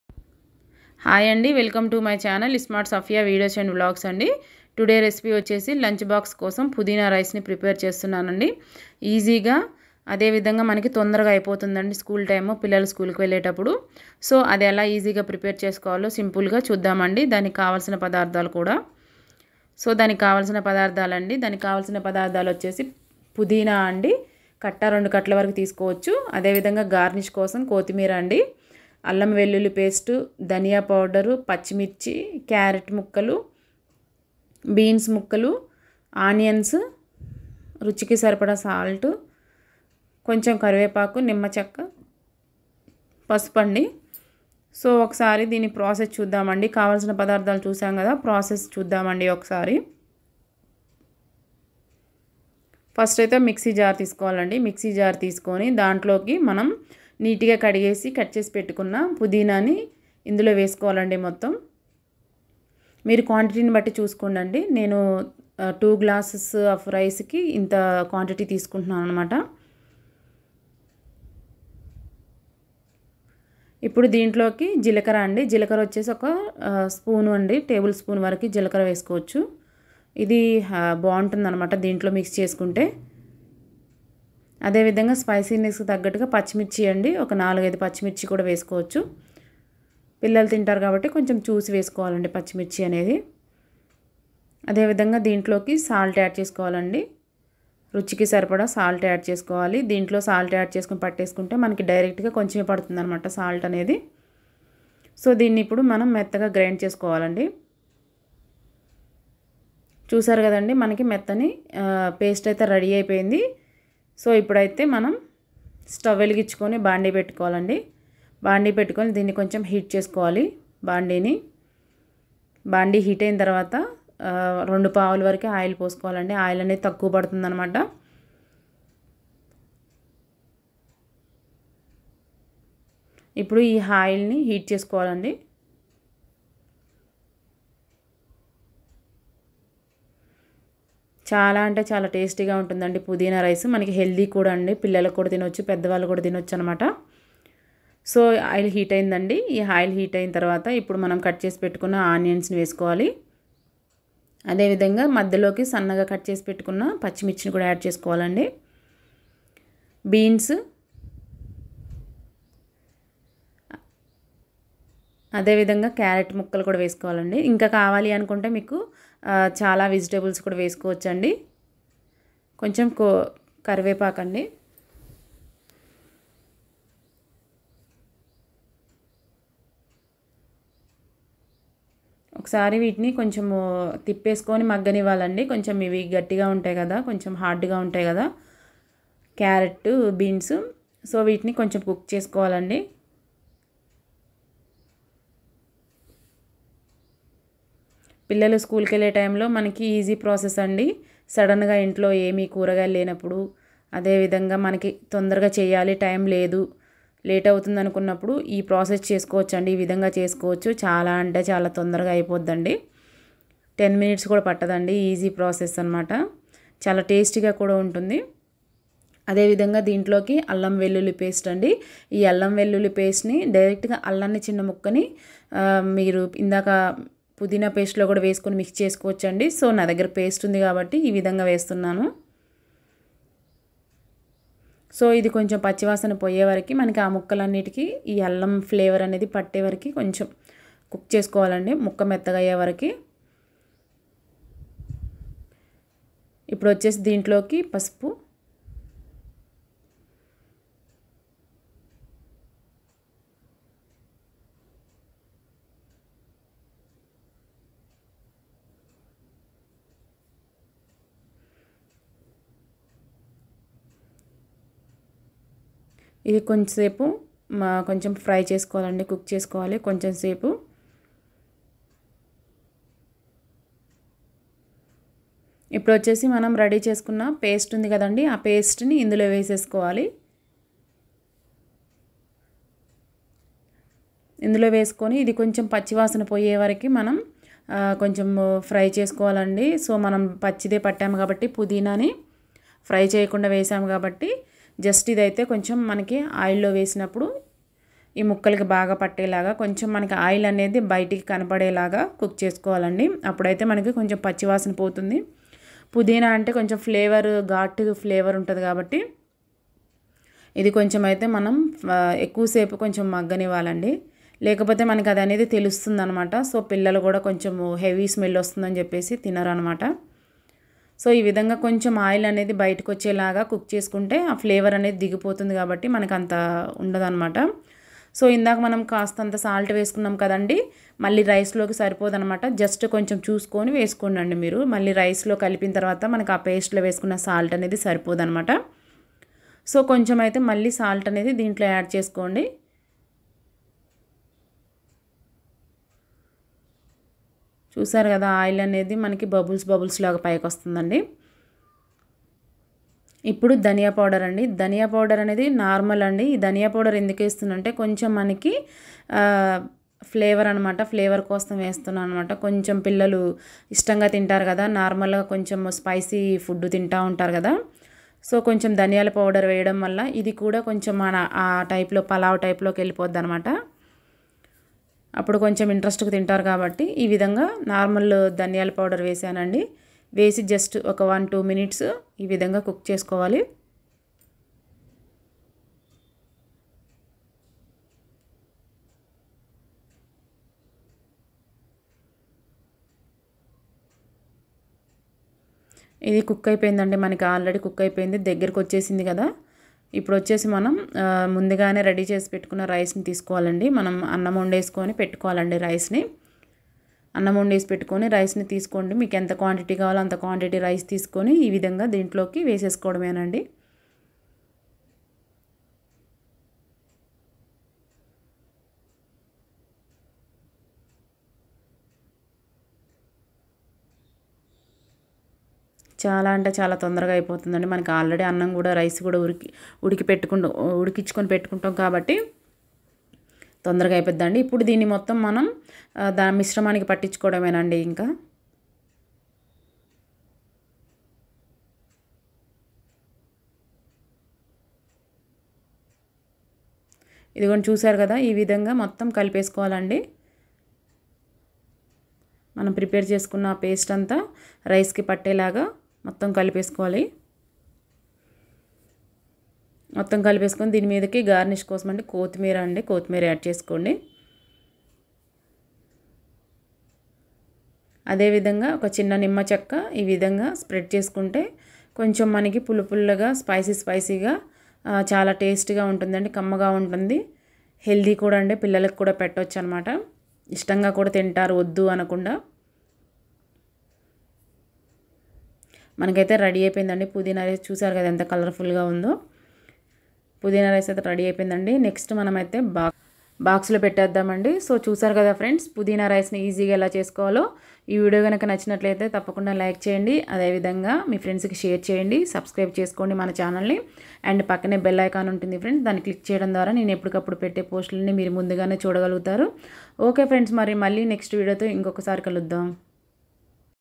పిల్లలకు ఎంతో ఇష్టమైన లంచ్ బాక్స్ రైస్ పుదీనా రైస్ ఇలా చేశారంటే కడుపునిండా తినాల్సిందే 👌👌
plz follow  in my channel

|| ismart sariga videos & vlogs ||